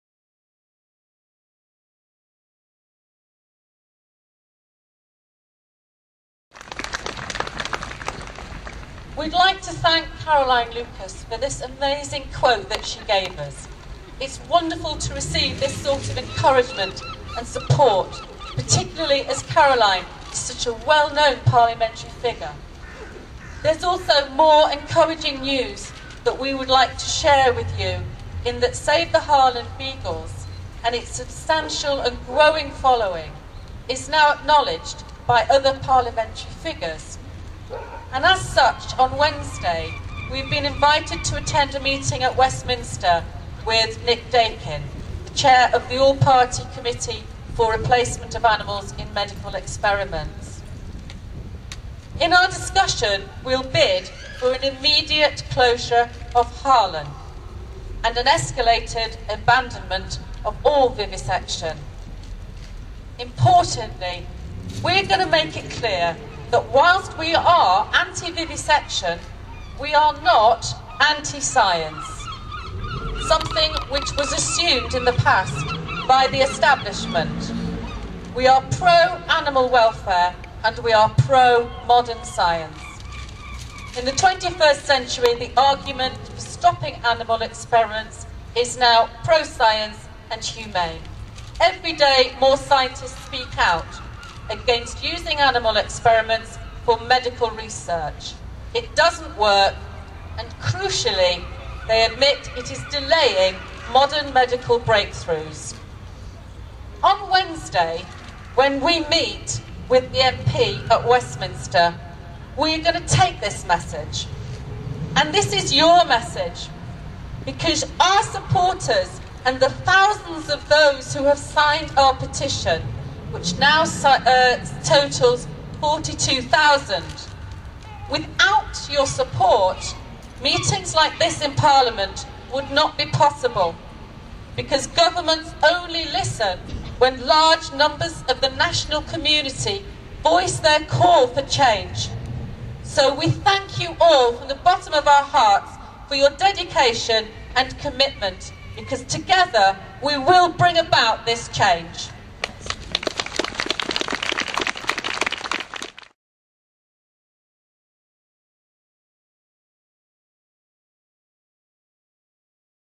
SAVE THE HARLAN BEAGLES - Brighton (UK), 09.03.2013 - Intervento di ALEX IRVING
La bravissima Alex Irving, membro del comitato organizzatore di Save The Harlan Beagles, ha portato testimonianza da parte del Gruppo dell'importanza del sostegno di tutti coloro che hanno firmato la petizione contro la vivisezione, grazie ai quali ben 42.000 firme sono state raccolte e sono state portate al Parlamento per avviare un dialogo produttivo con il governo al fine di porre fine alla sperimentazione animale nel Regno Unito.